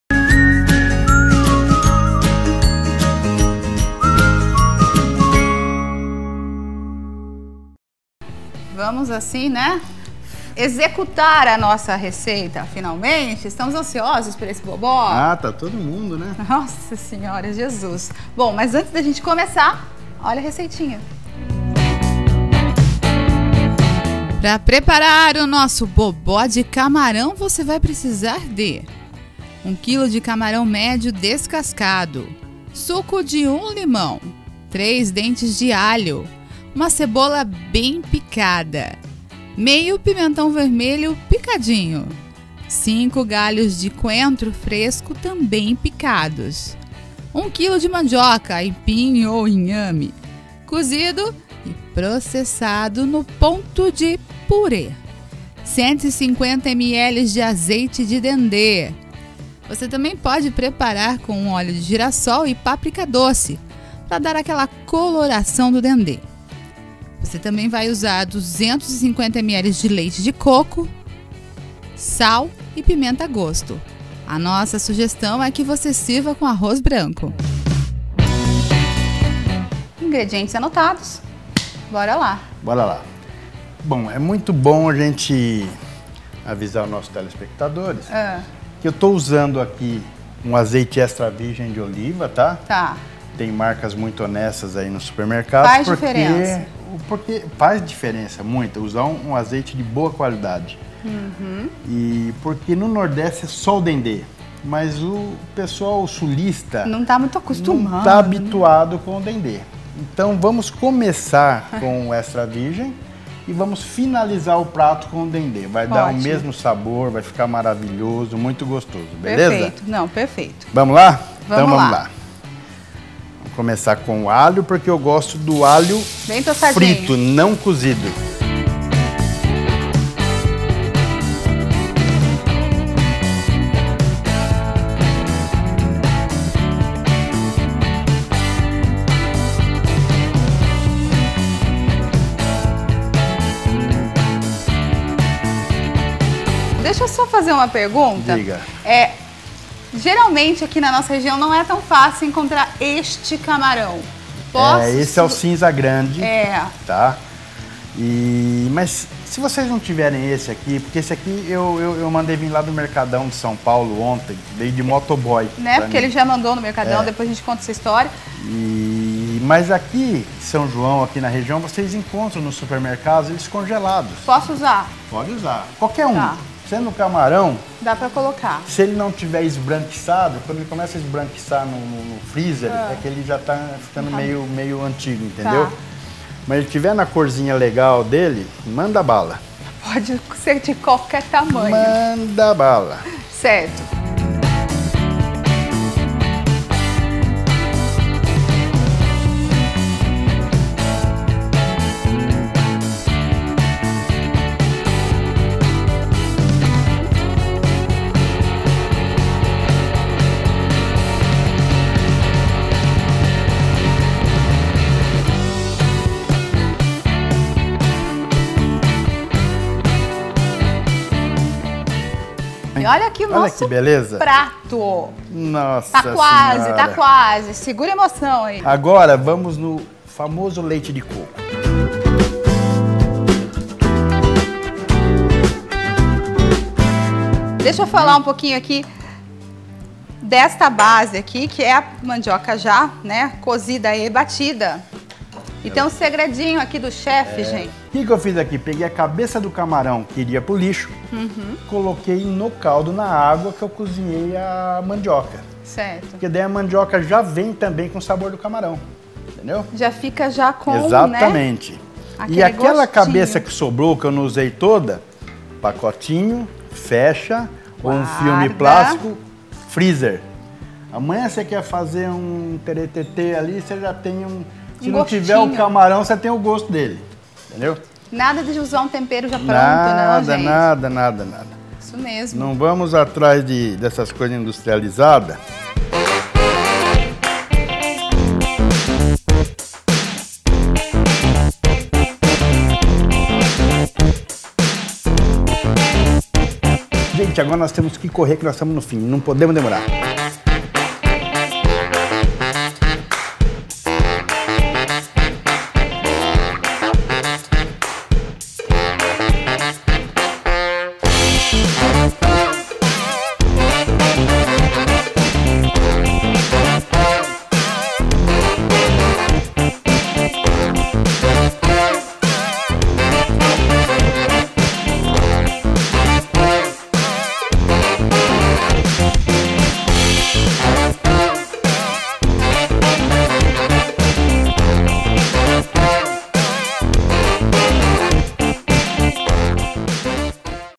RECEITA BOBÓ CAMARÃO
Receita de Bobó de Camarão

Ingredientes:
1 kg de camarão médio descascado
1 limão
3 dentes de alho
1 cebola
1/2 pimentão vermelho
5 galhos de coentro fresco
1 kg de mandioca, aipim ou inhame - Cozido e processado no ponto de purê
150 ml de azeite de dendê ou preparar com óleo de girassol e paprica doce para dar a coloração do dendê
250 ml de leite de coco

Serve com Arroz branco

Modo de preparo:

1. Em um recipiente adequado, temperar os camarões com sal, limão espremido, coentro picado, alho, cebola e pimentão vermelho bem picadinho.
2. Aquecer uma panela com o azeite de dendê (ou similar), juntar os camarões com todos os outros ingredientes e deixar cozinhar por 10 minutos.
3. Misturar então com o purê de mandioca, mexer bastante e adicionando o leite de coco aos poucos até ficar com textura de purê.
4. Sirva imediatamente com arroz branco de guarnição.

Bom apetite!!